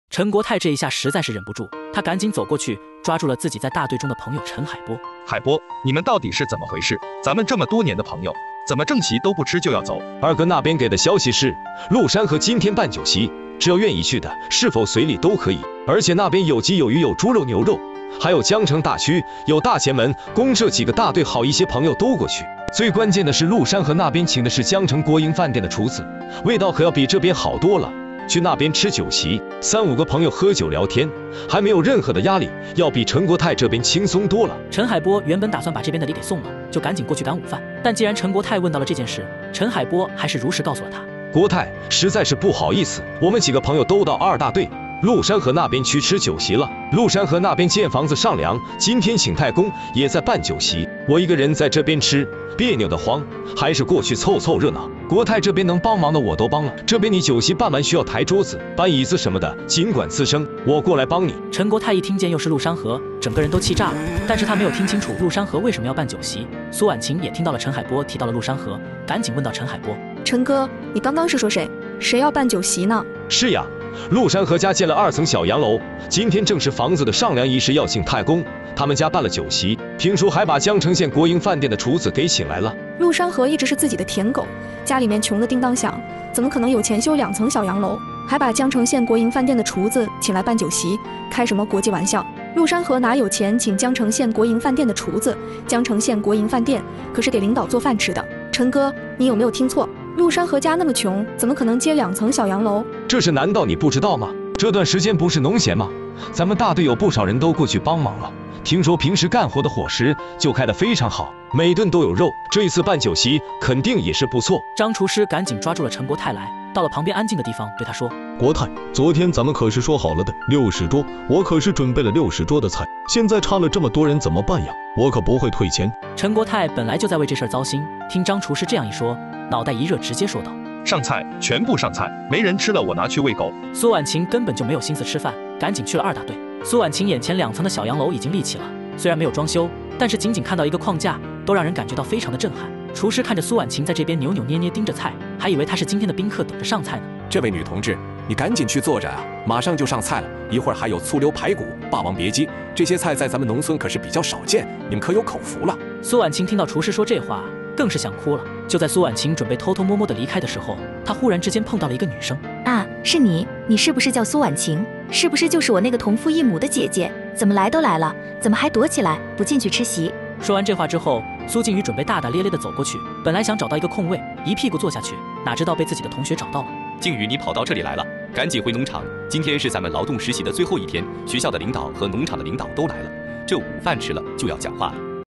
#88 重生回到80年代，蘇婉晴看到陸山河的房子，被震驚的說不出話#原創動畫 動畫製作【兔先生】
我們強調了影片內容的合規性，絕對沒有任何不良引導或危險行為。
我们将在这里与您分享有趣的动画作品
个人做动画，请大家多多支持~